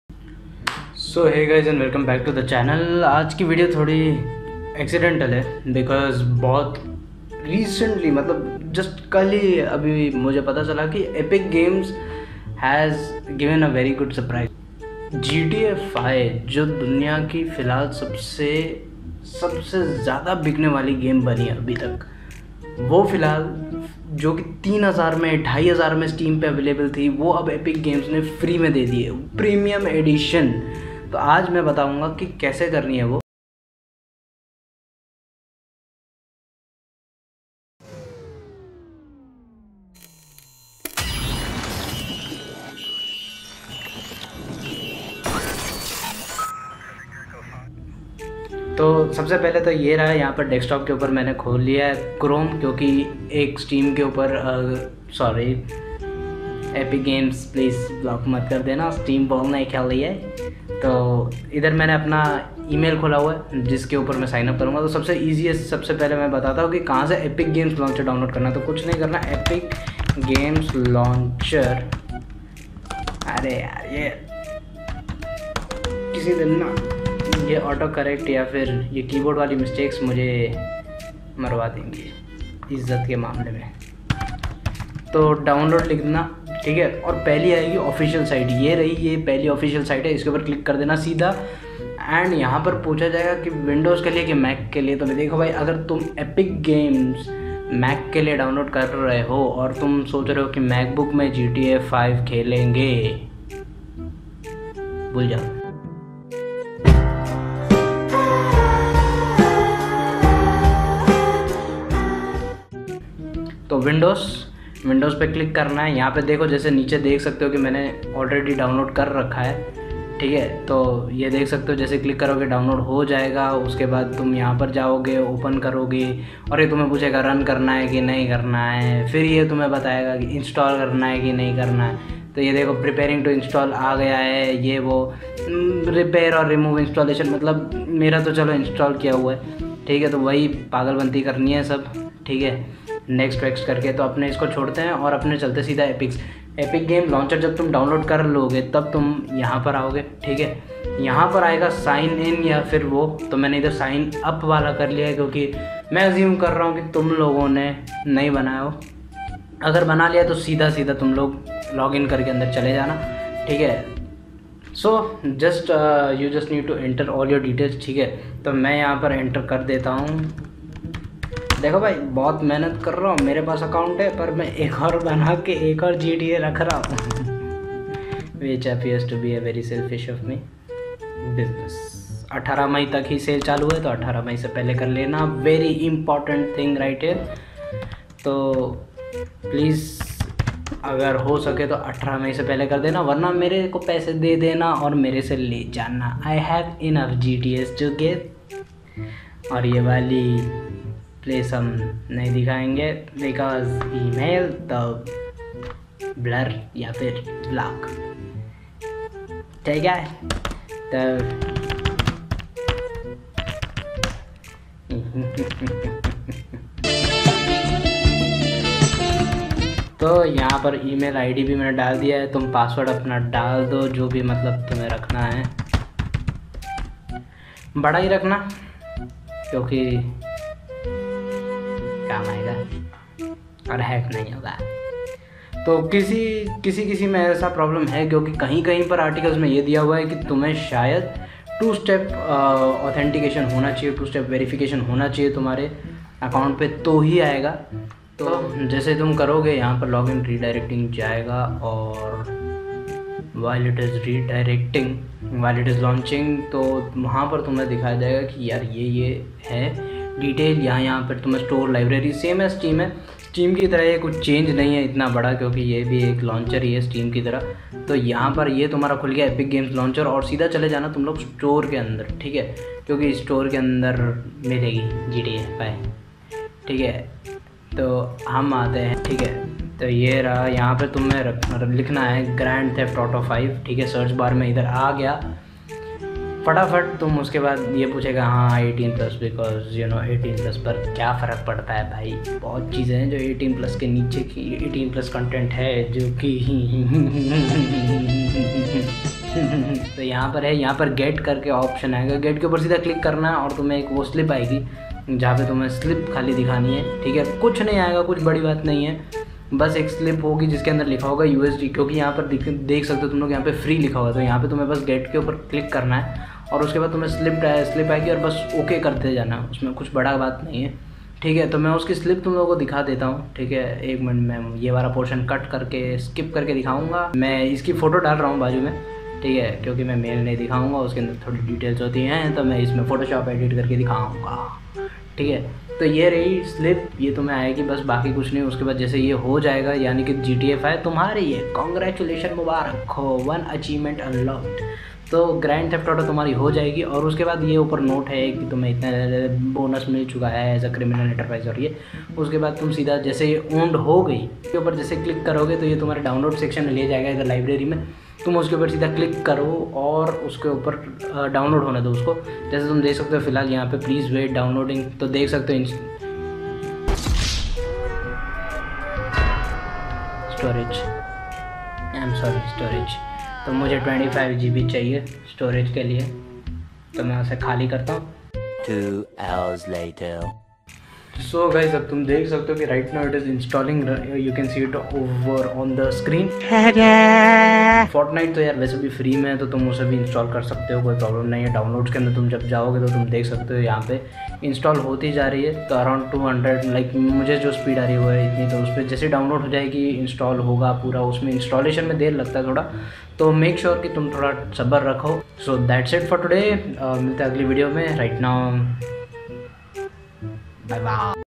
Purchase GTA 5 PREMIUM EDITION for FREE | EPIC GAMES OFFER
GTA is the game every teenager has played and its loved by all which has made GTA5 the most sold game in the world till now.
GTA 5 PREMIUM EDITION which allows to play online as well as offline.. it was sold for around 2000 INR on platforms but now is free till 21 MAY 2020 on epic games... check video to know the process to get yours!